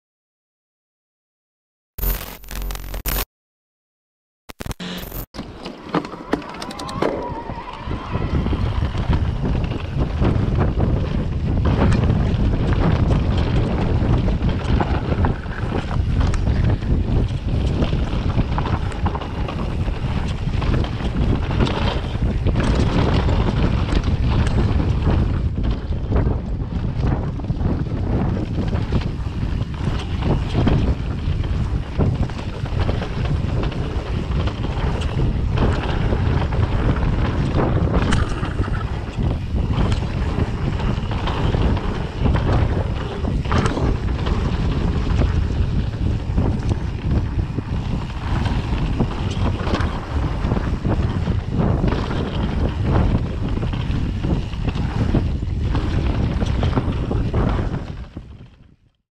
Krecik DH - Bajkowe - E-bike
38 singli XC/Trail/Enduro o łącznej długości ponad 17 km, pięknie położonych w samym centrum Trójmiejskiego Parku Krajobrazowego na terenie Sopot i Gdańsk Oliwa.